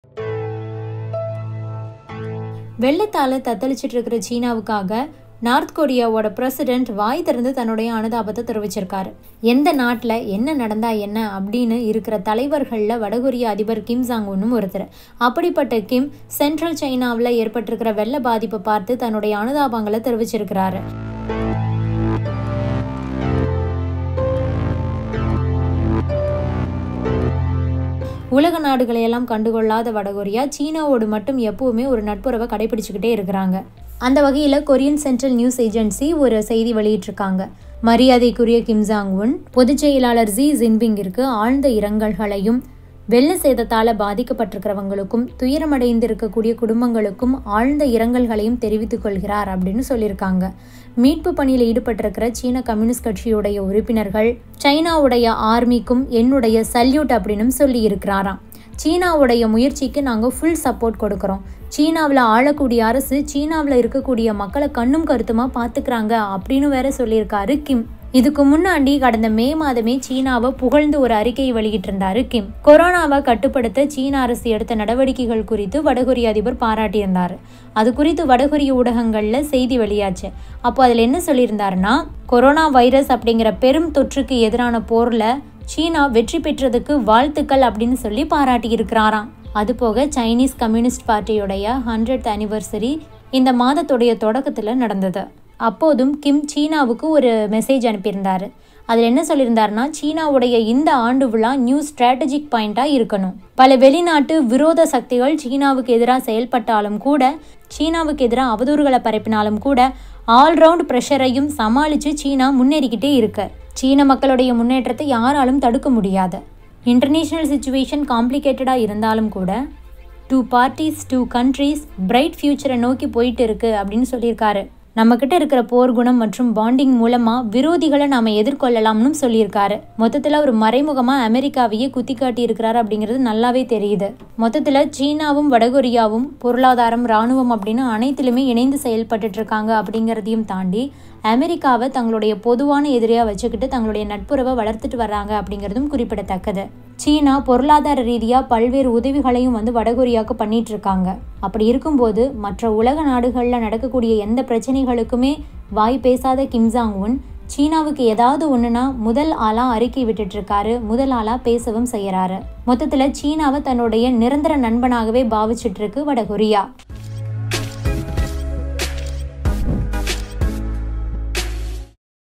China-வை விடாத North Korea | China Flood | China-North Korea Relationship | Oneindia Tamil
Kim Jong Un sends message of sympathy to Xi Jinping over floods

வெள்ளத்தால் தத்தளிக்கும் சீனாவுக்காக North Korean leader Kim Jong Un வாய்திறந்து தனது அனுதாபத்தை தெரிவித்துள்ளார்.